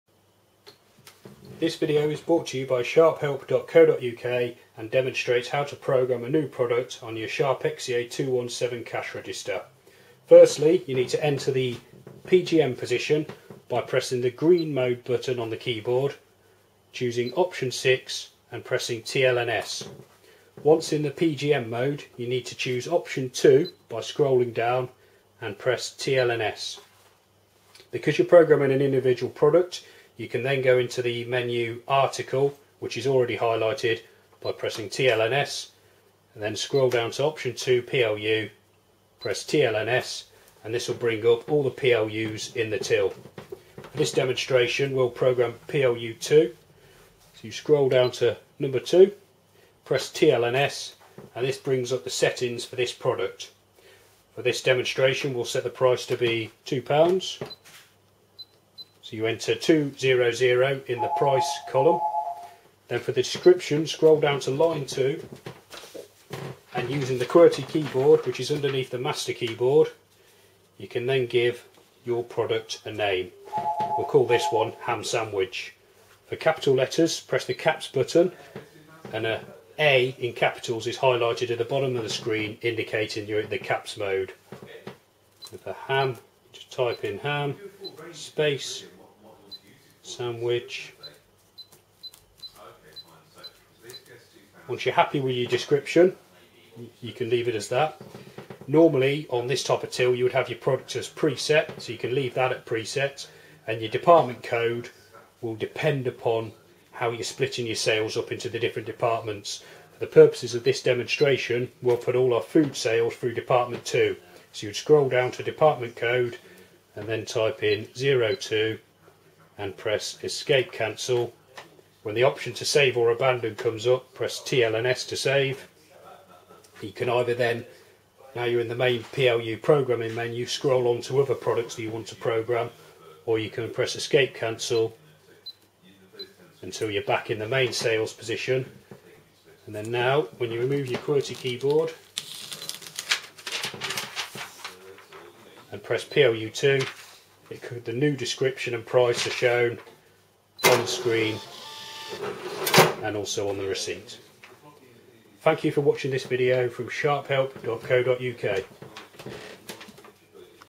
How to program a product on a Sharp XE-A217 Cash Register
How to program a product into a Sharp XE-A217 Cash Register.